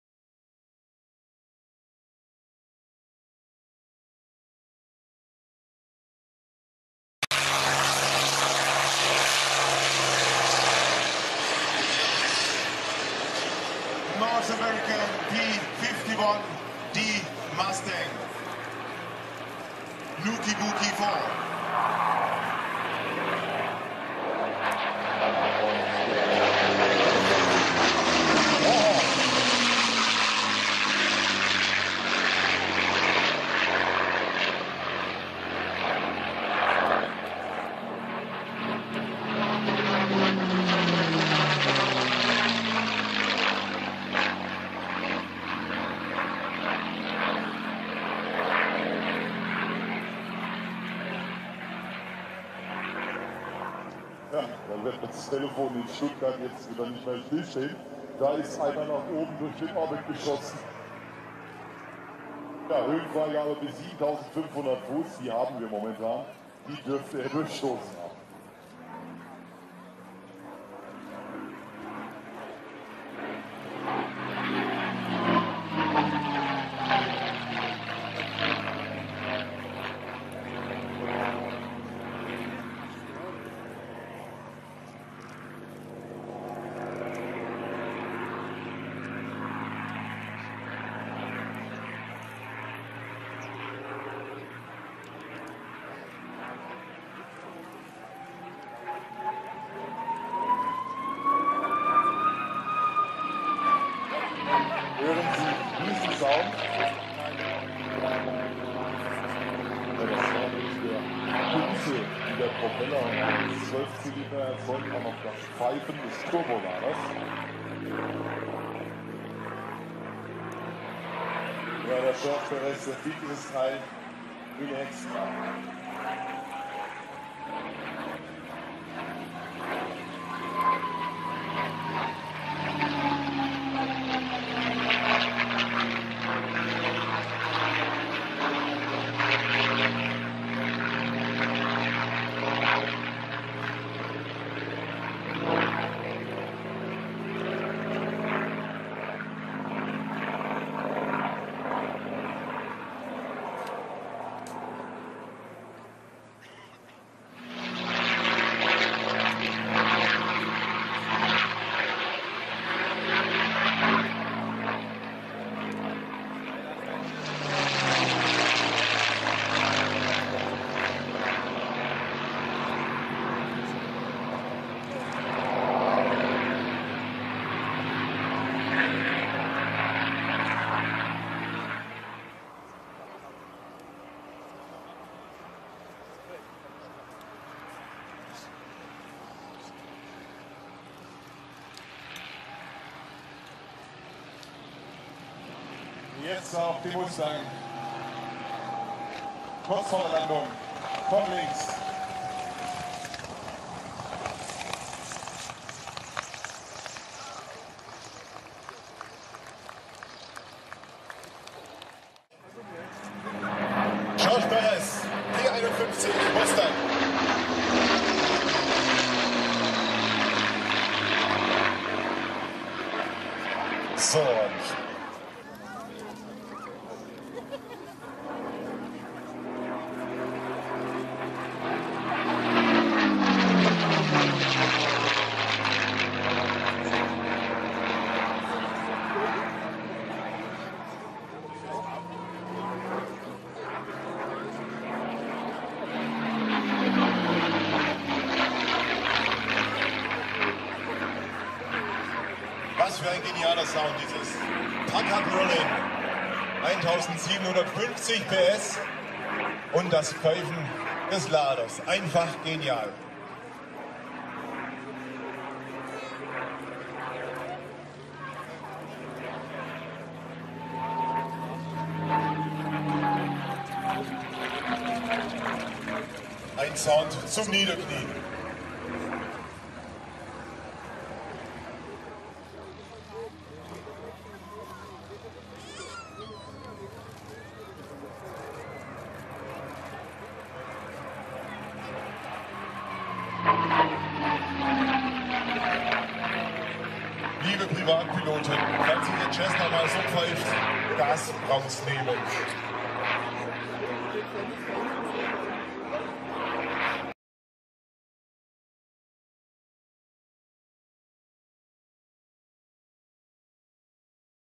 North American P-51 Mustang - Hahnweide Air Show 2013
The North American P-51 Mustang manufactured by North American Aviation is an American long-range, single-seat fighter and fighter-bomber aircraft. The P-51 Mustang was the first U.S. built fighter airplane to push its nose over Europe after the fall of France. The P-51 Mustang was the first single-engine plane based in Britain to penetrate Germany, first to reach Berlin, first to go with the heavy bombers over the Ploiesti oil fields in Romania, and first to make a major-scale, all-fighter sweep specifically to hunt down the dwindling Luftwaffe. The P-51 Mustang also saw its action during World War II and during the Korean War. The maiden flight of the P-51 Mustang took place on the 26th of October 1940 and it went into service on January 1942. Primary users were the United States Army Air Forces, Royal Air Force and Chinese Nationalist Air Force. There were more than 15,000 P-51 Mustang produced. The P-51 Mustang retired from military service 1984. Cost per unit was approx. 50,985 USD in 1945.